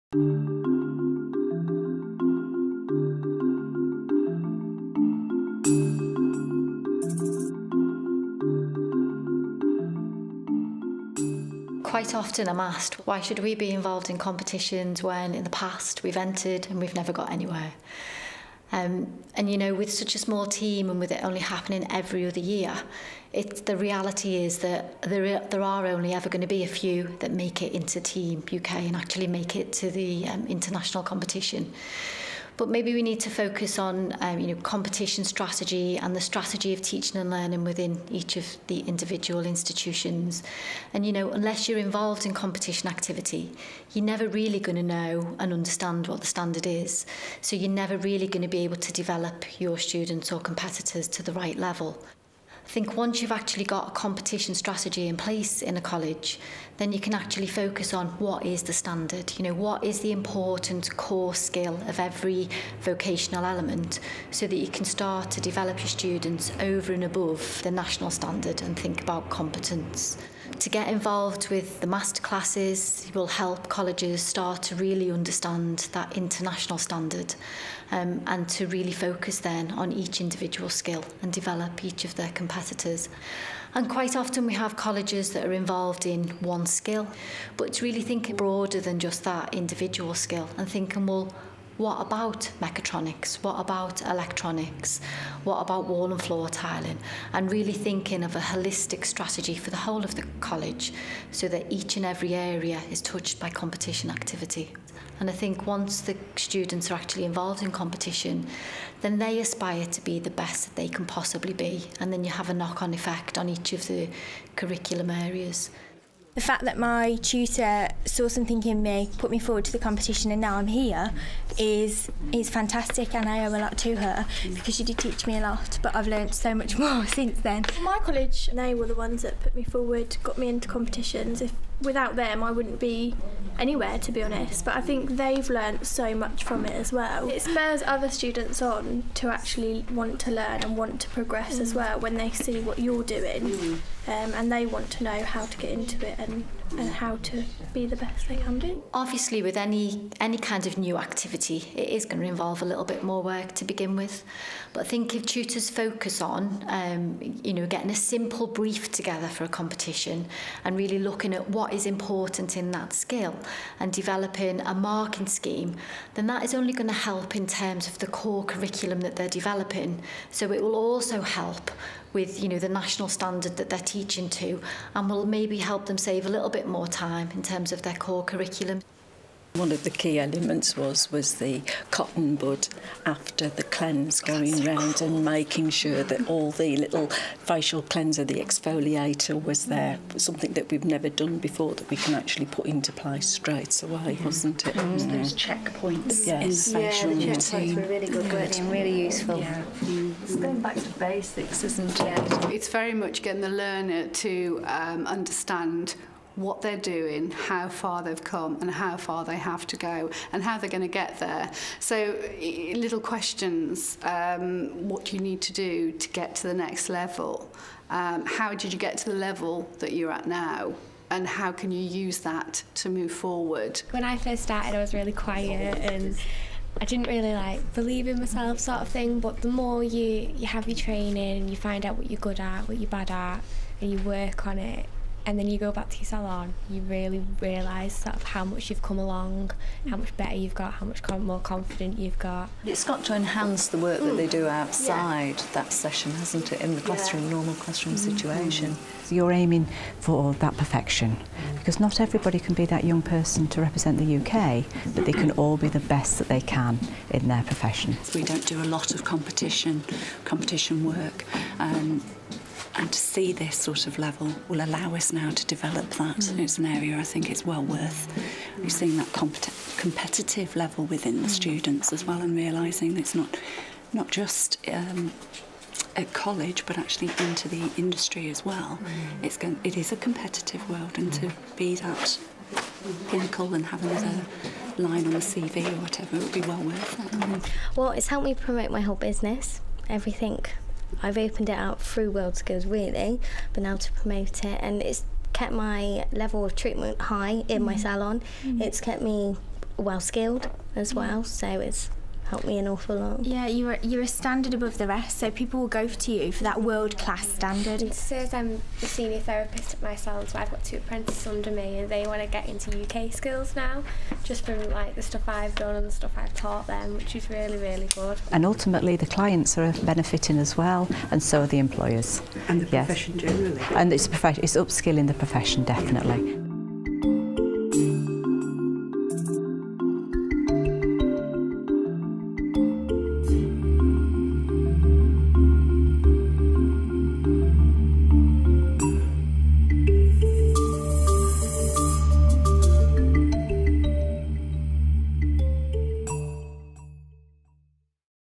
Raising The Bar
This video is to inform of reasons to enter WorldSkills and the competitions, and the benefits you can gain from it.